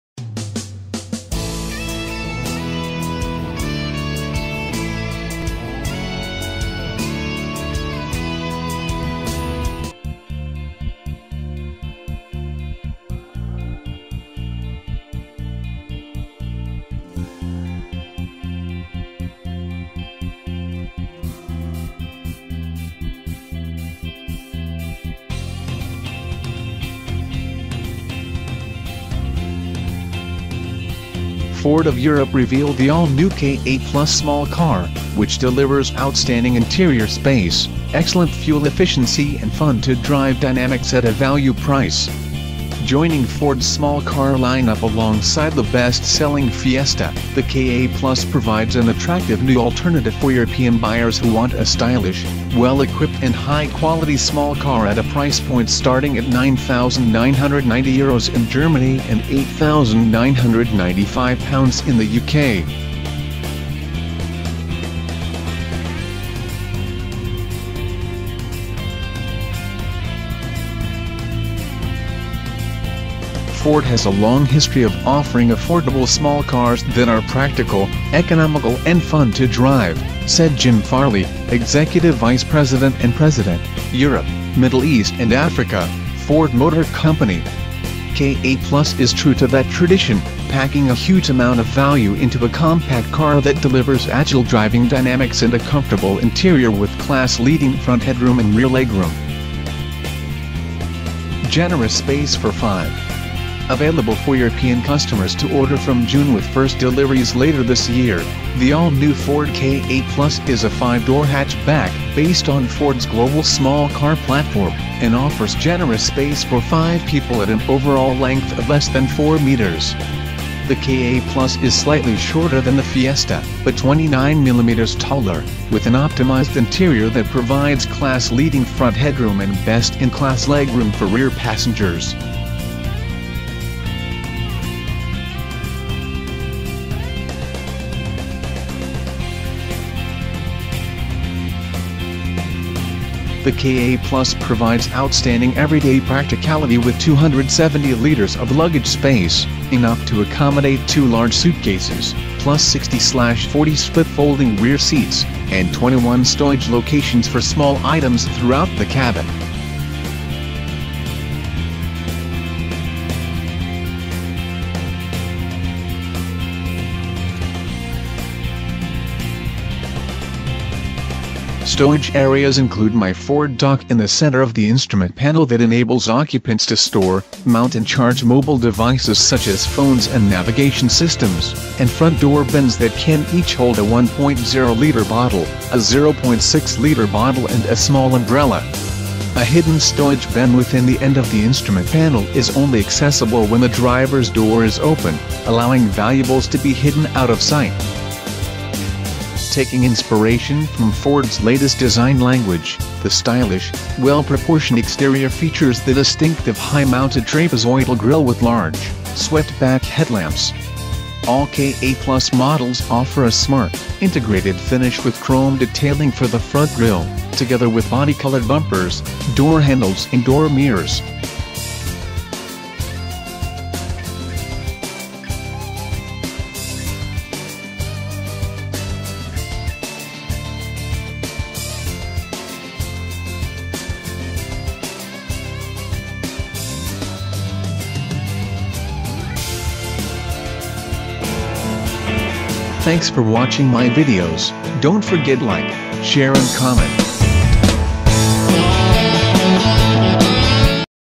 Latest Autovlog News : Ford Ka plus 2017
Ford of Europe revealed the all-new KA+ small car, which delivers outstanding interior space, excellent fuel efficiency and fun-to-drive dynamics at a value price.

Joining Ford's small-car line-up alongside the best-selling Fiesta, the KA+ provides an attractive new alternative for European buyers who want a stylish, well-equipped and high-quality small car at a price point starting at €9,990 in Germany and £8,995 in the U.K.

"Ford has a long history of offering affordable small cars that are practical, economical and fun-to-drive," said Jim Farley, executive vice president and president, Europe, Middle East and Africa, Ford Motor Company. "KA+ is true to that tradition, packing a huge amount of value into a compact car that delivers agile driving dynamics and a comfortable interior with class-leading front headroom and rear legroom."